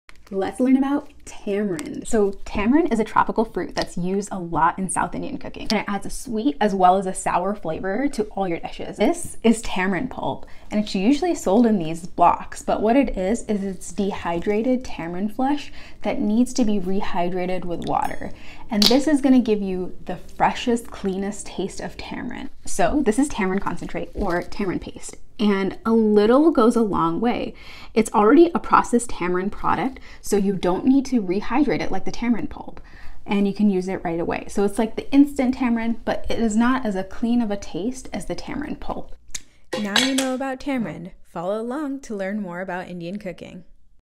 What is tamarind?!
I personally use tamarind to flavor my rasam's and sambars. You can learn more about tamarind and south indian dishes that use it on the blog!